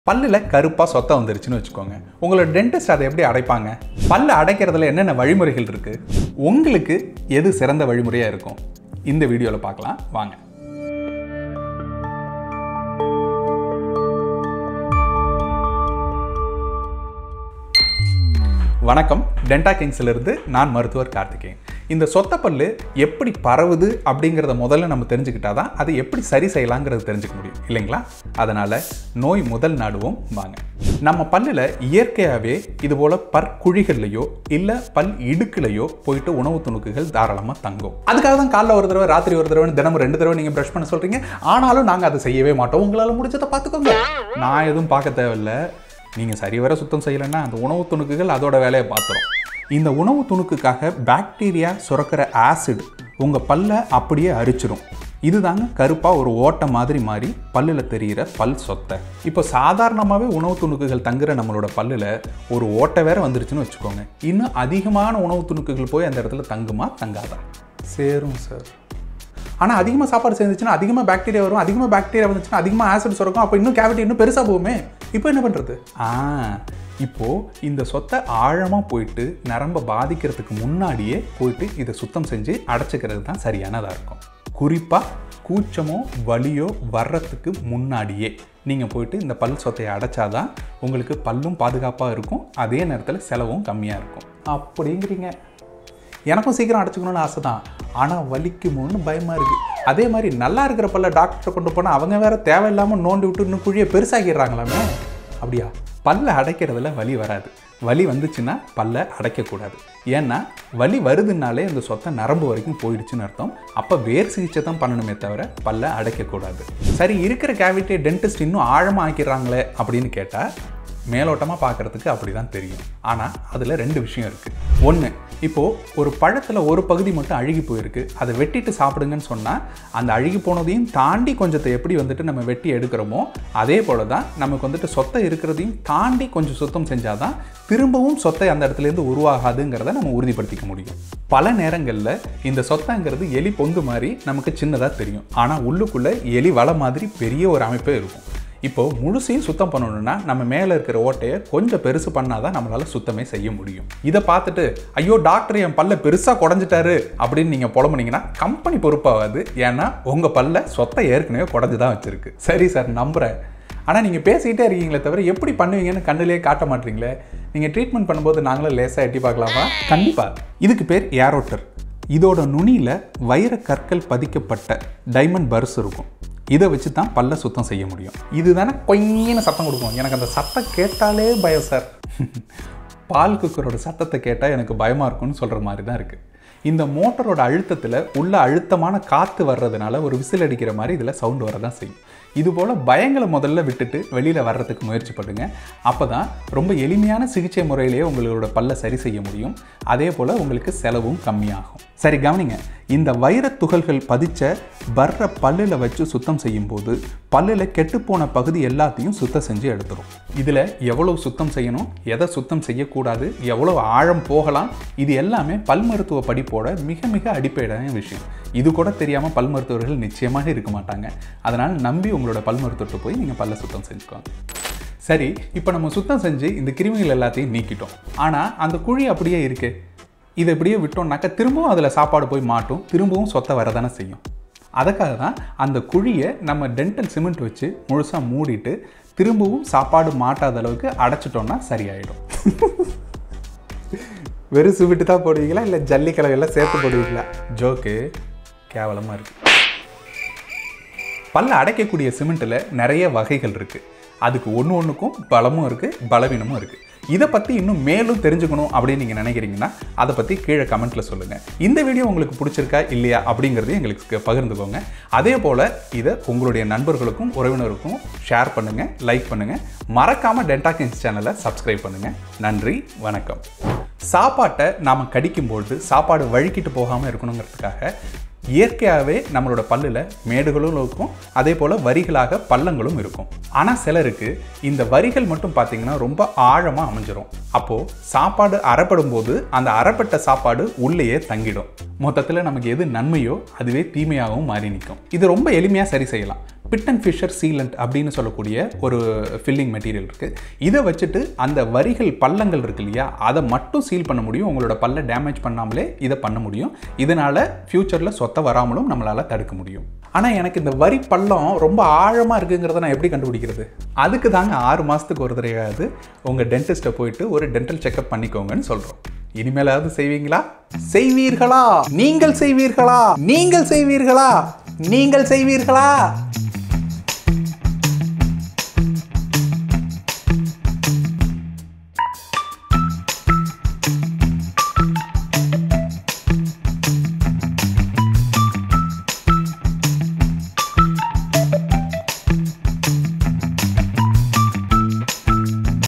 How dentists fill cavities - சொத்தை பல்லை உங்கள் பல் மருத்துவர் எப்படி அடைக்கிறார்? | Denta Kings
Dr. Karthikeyan from Denta Kings Dental Implant Centre, Madipakkam, Chennai explains about the process of cleaning tooth cavity, and how the cavity area is Filled after the cleaning. From this video you can also get clarity on some common misconception on treatment for Cavity and Filling.